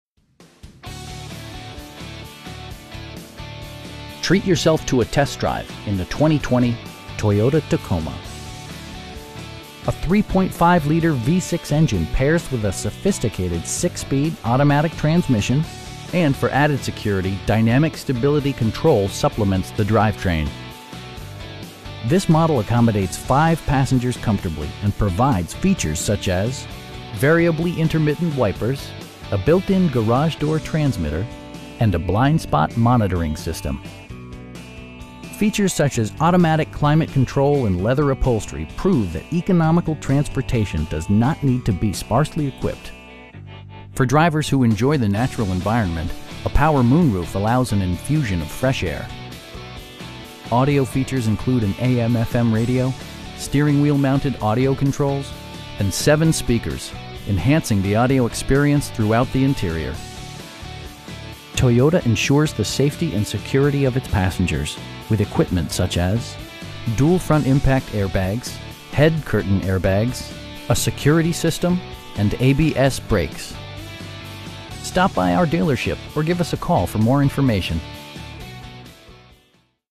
2020 Toyota Tacoma 4WD Limited in Merriam, KS 66203
9505 W. 67th Street

Treat yourself to a test drive in the 2020 Toyota Tacoma. A 3.5 liter V-6 engine pairs with a sophisticated 6 speed automatic transmission, and for added security, dynamic Stability Control supplements the drivetrain. Four wheel drive allows you to go places you've only imagined. This model accommodates 5 passengers comfortably, and provides features such as: a tachometer, variably intermittent wipers, a built-in garage door transmitter, front fog lights, heated door mirrors, power windows, and a blind spot monitoring system. Features such as automatic climate control and leather upholstery prove that economical transportation does not need to be sparsely equipped. For drivers who enjoy the natural environment, a power moon roof allows an infusion of fresh air. Audio features include an AM/FM radio, steering wheel mounted audio controls, and 7 speakers, providing excellent sound throughout the cabin. Toyota ensures the safety and security of its passengers with equipment such as: dual front impact airbags, head curtain airbags, traction control, brake assist, anti-whiplash front head restraints, a security system, an emergency communication system, and ABS brakes. Adaptive cruise control maintains a pre set distance behind the car ahead of you, simplifying highway driving and enhancing safety. We pride ourselves in consistently exceeding our customer's expectations. Stop by our dealership or give us a call for more information.